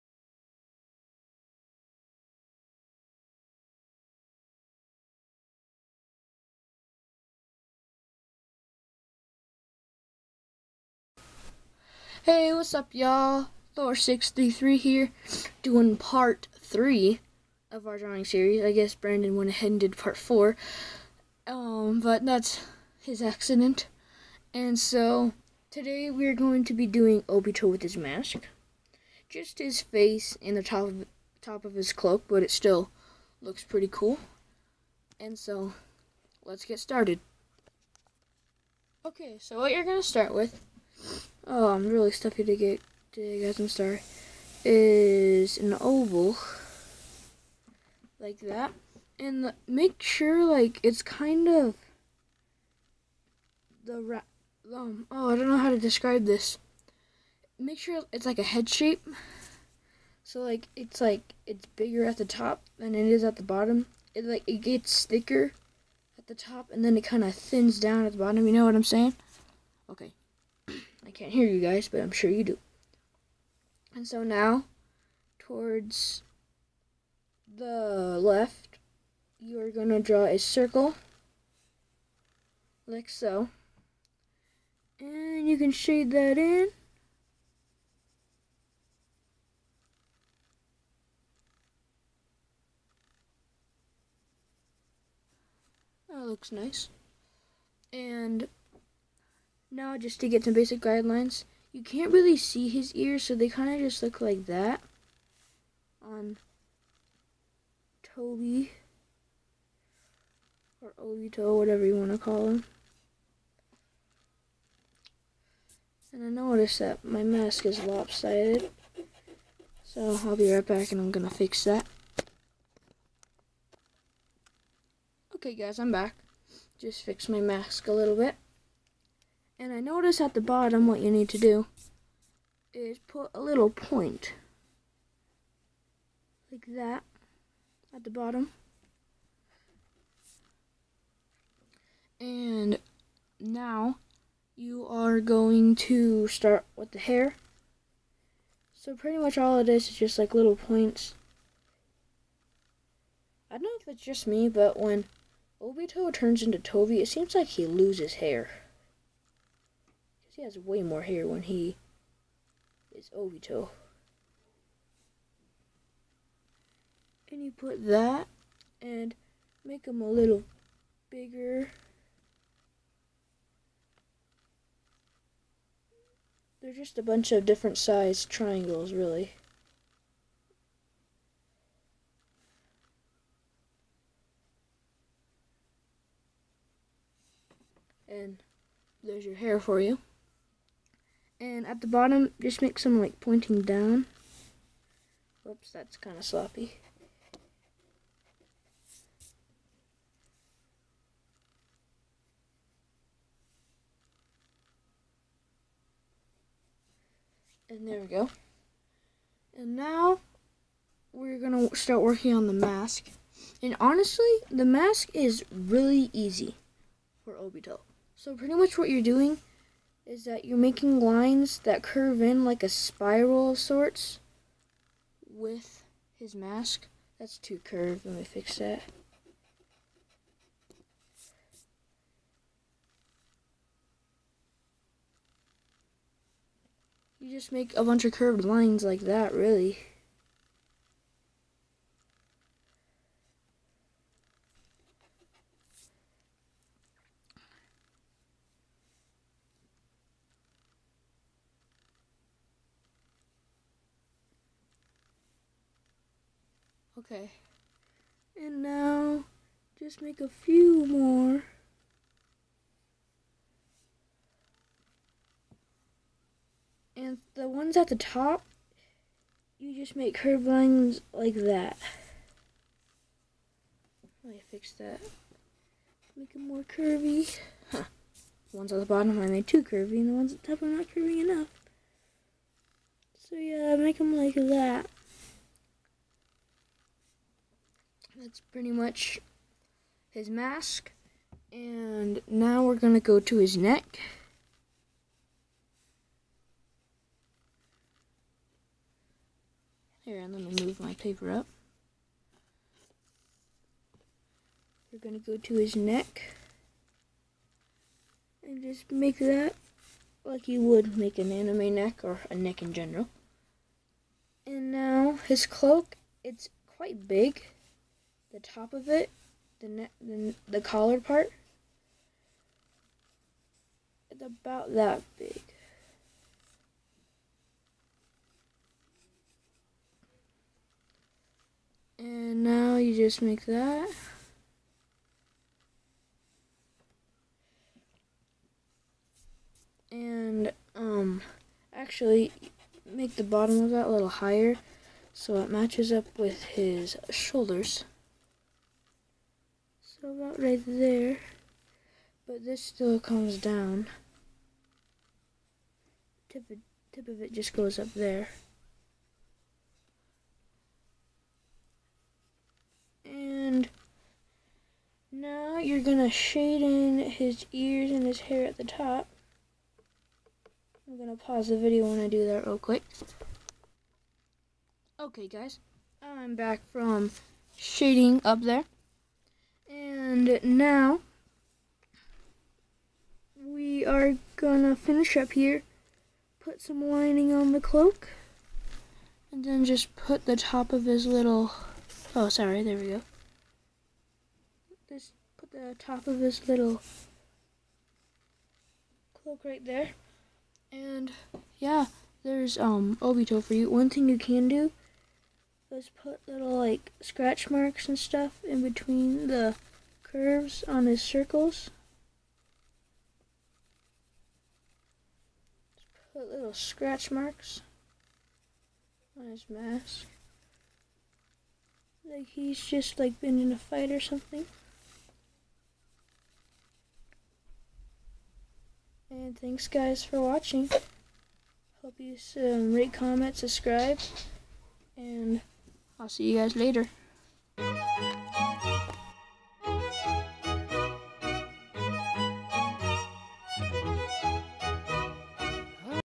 How to Draw Obito
Today were doing Obito with his mask, ENJOY!